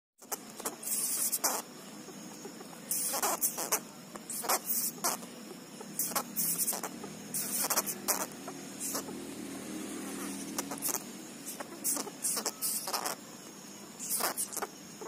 Sugar Glider Mom Play With Her Joeys (4 week old baby)
4 week old baby